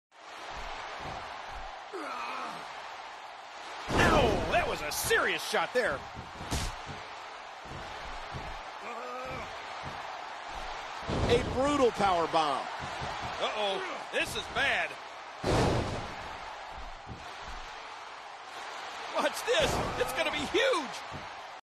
Juvetud Guerrera's Finishers & Signatures
Juvetud Guerrera's Finishers & Signatures sig- 911 Juvi Driver & Juvi Driver fin- Torture Rack Powerbomb & 450 Splash
psn-mcr2469